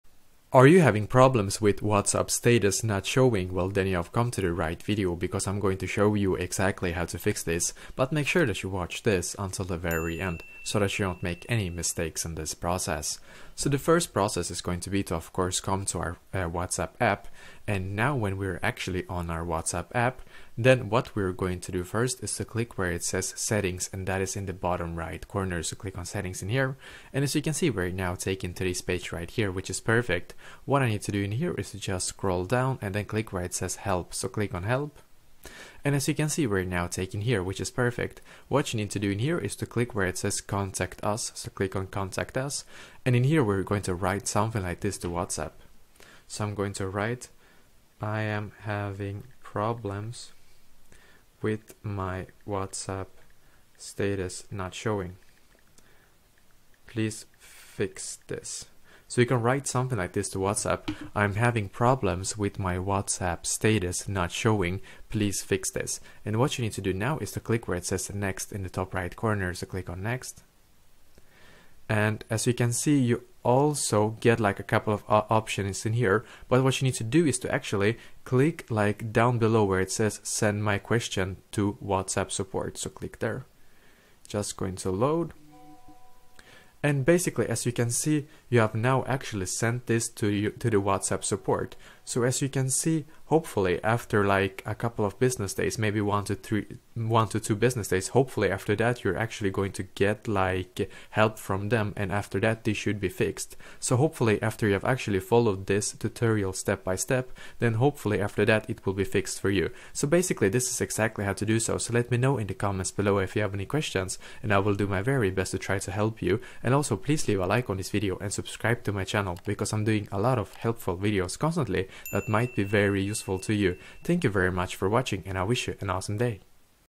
WhatsApp Status Not Showing (FIX)
If you want to be able to fix WhatsApp status not showing then this video will be perfect for you!